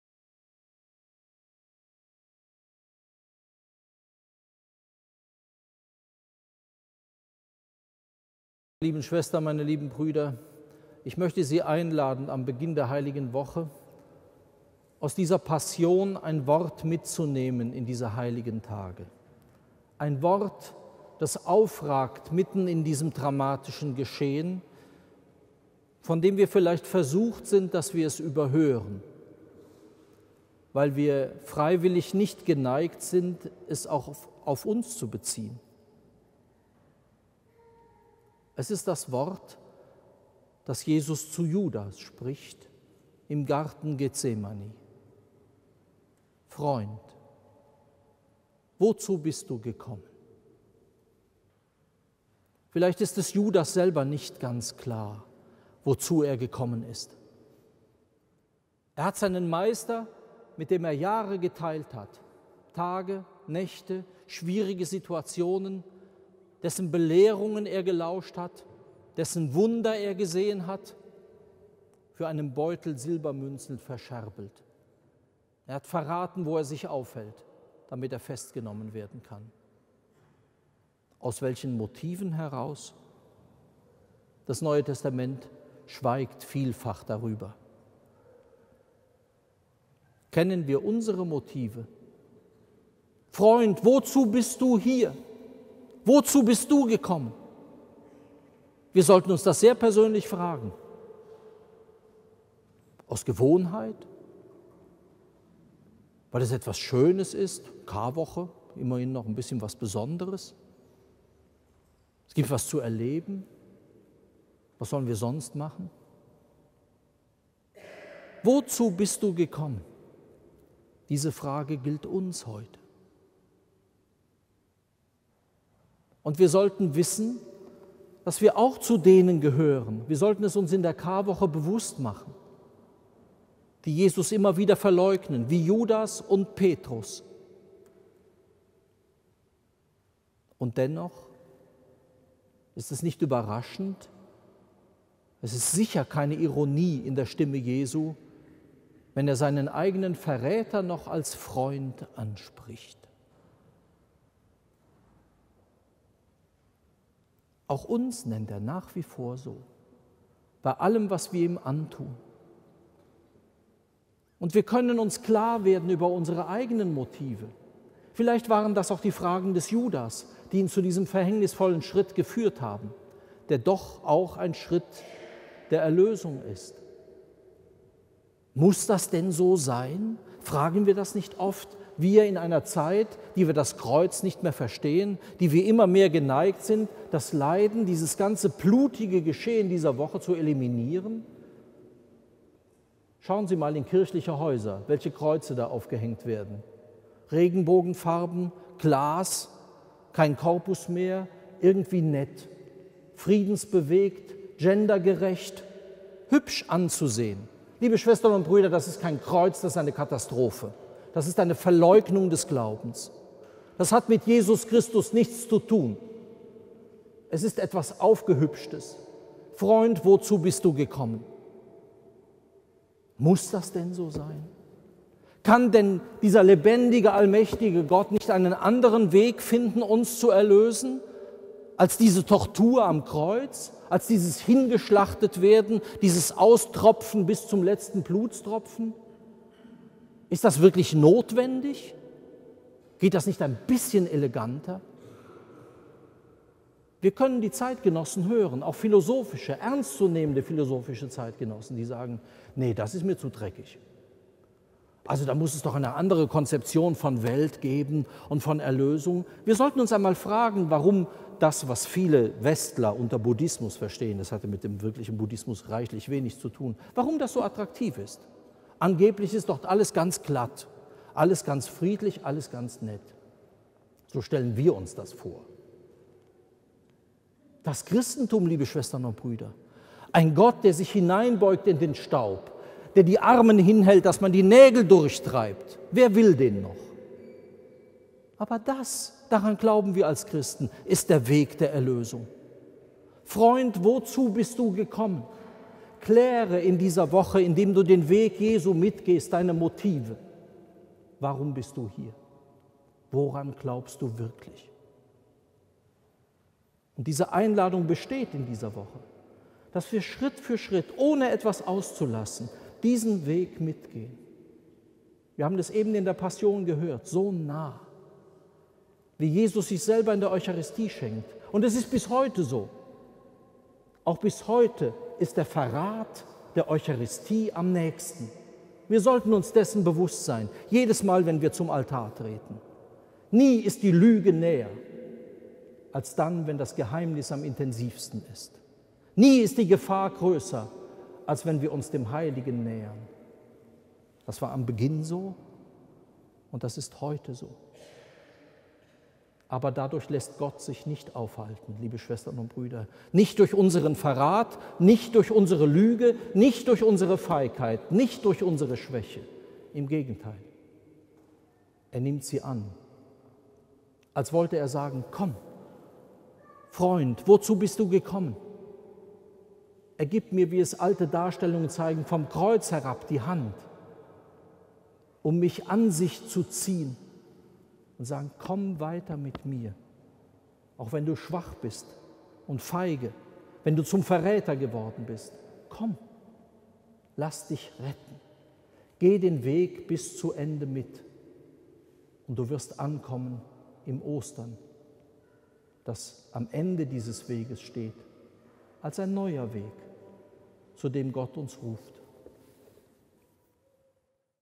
Freund, wozu bist du gekommen?
Predigt an Palmsonntag von Rektor Dr. Johannes J. Kreier im Kloster am Rande der Stadt/ SB 2017.